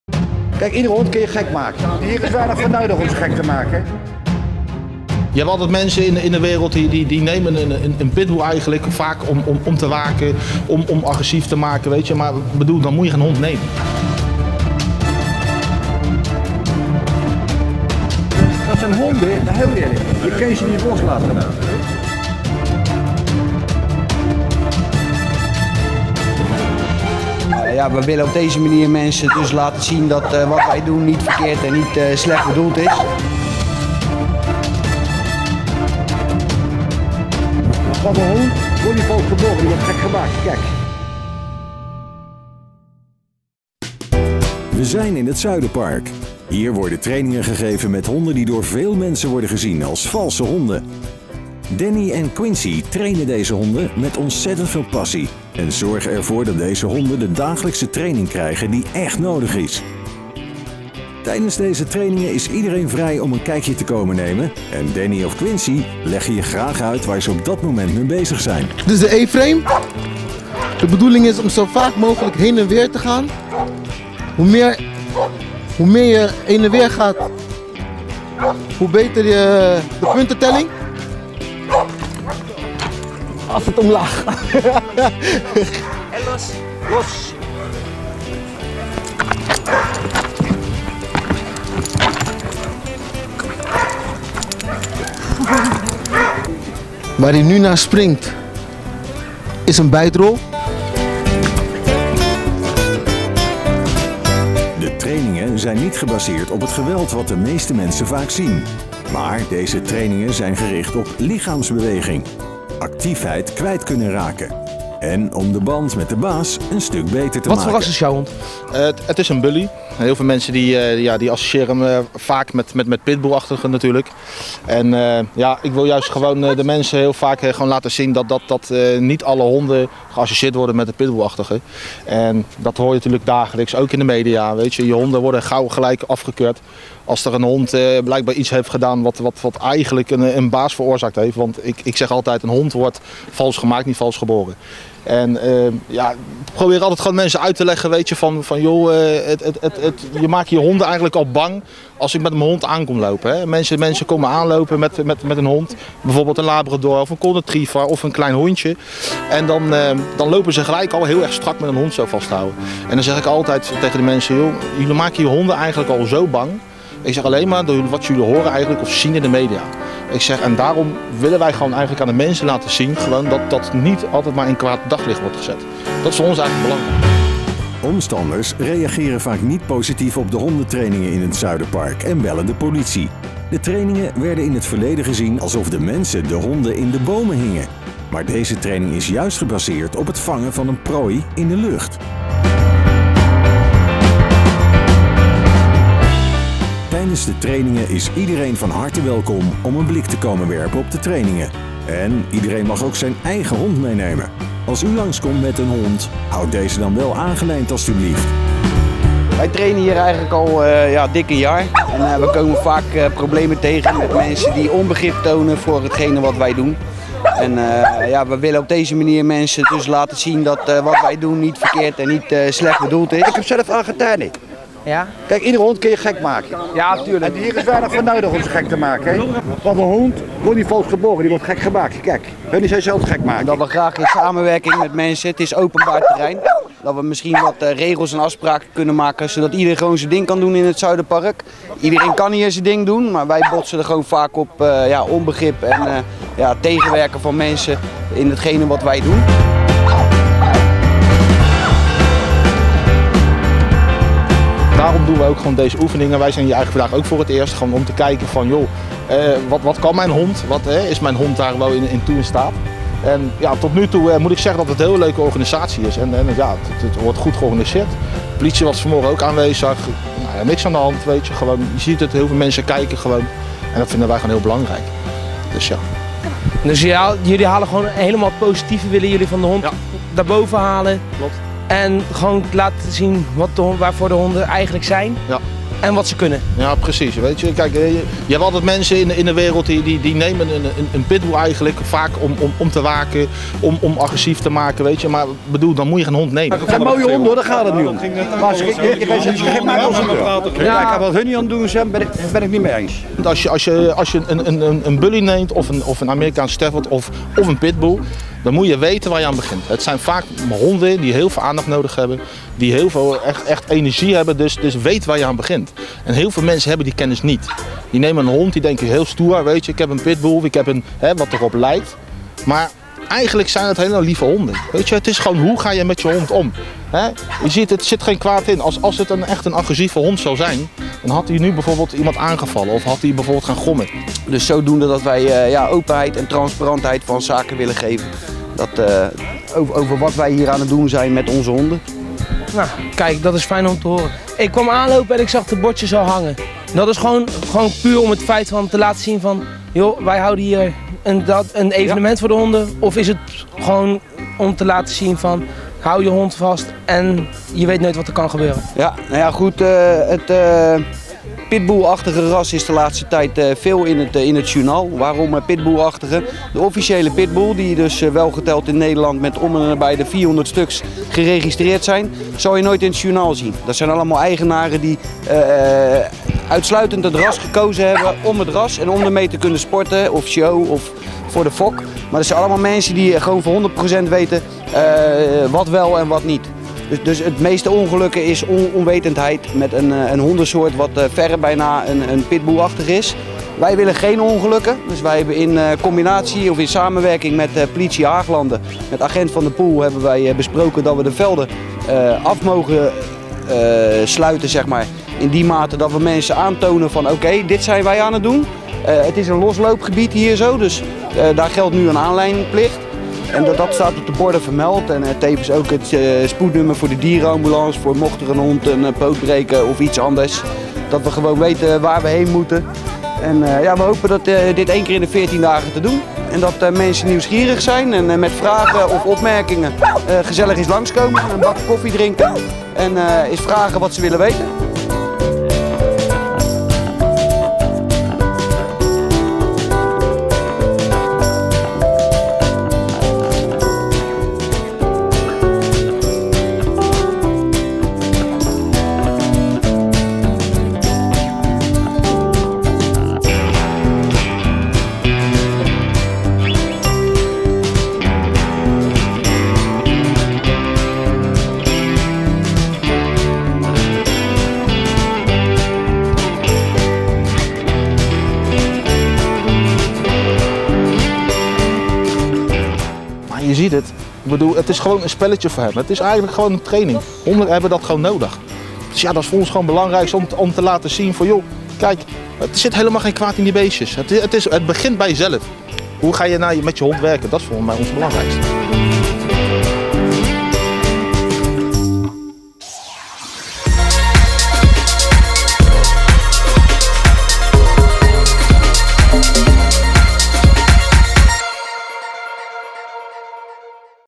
WTF TV - Dit kan je allemaal doen met een Powerbreed
We zijn in het Zuiderpark.  Hier worden trainingen gegeven met honden die door veel mensen gezien worden als valse honden.
Dennis en Quince trainen deze honden met ontzettend veel passie en zorgen ervoor dat deze honden de dagelijkse training krijgen die echt nodig is.

Tijdens deze trainingen is iedereen vrij om een kijkje te komen nemen en Dennis of Quince leggen je graag uit waar ze op dat moment mee bezig zijn.

De trainingen zijn niet gebasseerd op het geweld wat de meeste mensen vaak zien, maar deze trainingen zijn gericht op de lichaamsbeweging, actiefheid kwijt kunnen raken, en om de band met de baas een stuk beter te maken.

Omstanders reageren vaak niet positief op de hondentrainingen in het Zuiderpark en bellen de politie. De trainingen werden in het verleden gezien alsof de mensen de honden in de boom hingen, maar deze training is gebasseerd op het vangen van een prooi in de lucht.

Tijdens de trainingen is iedereen van harte welkom om een blik te komen werpen op de trainingen, en iedereen mag ook zijn eigen hond meenemen. Als u langskomt met een hond, houdt deze dan wel aangelijnd a.u.b. Zo zorgen we met zijn alle voor een goede veiligheid.